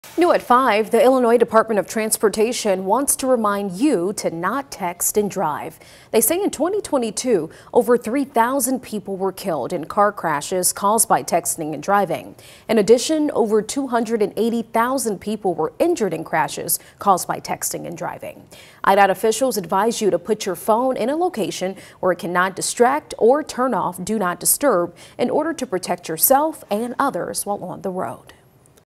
IDOT reminds drivers of texting and driving dangers
IDOT urges drivers to avoid texting and driving, citing dangers and recommending ways to minimize phone distractions.